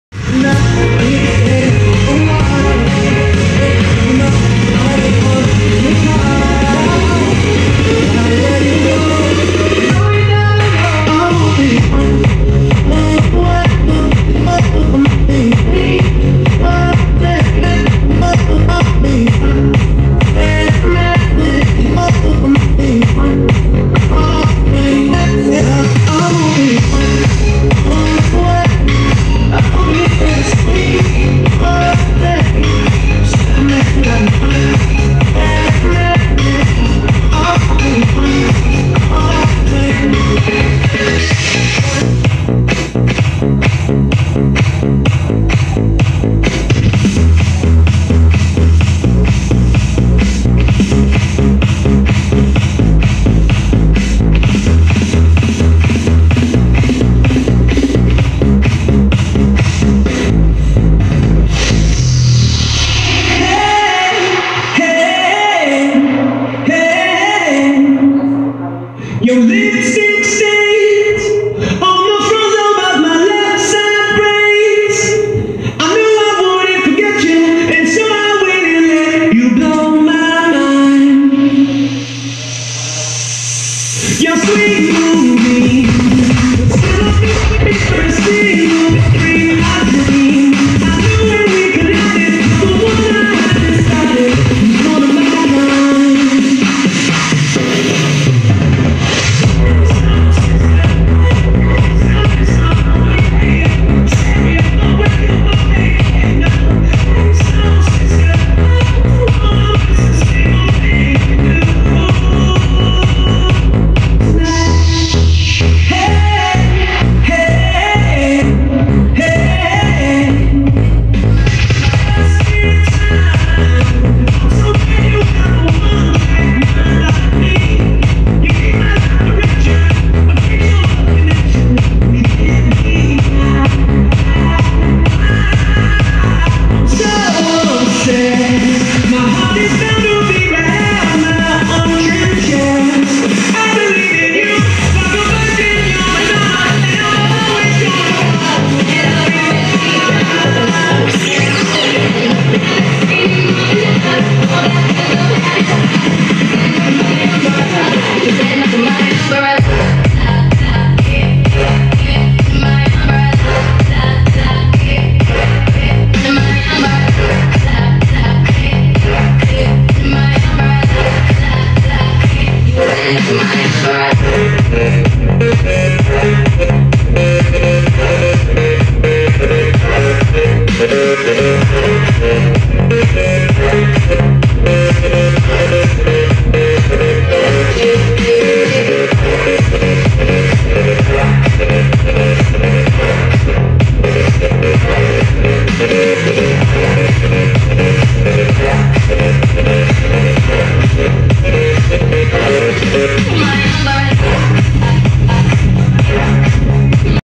Dj LøNg$kiÊ @ BerNz Birthday Party 2017
Just a short vid clip taken duRing my engagement @ Bernz birthday Party while the guests are eating 😋😁😎 6 October 2017